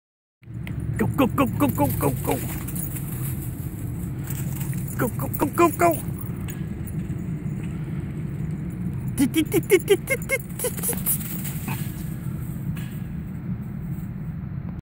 Greyhound / Whippet Dog Sprint
Dogs chasing after each other.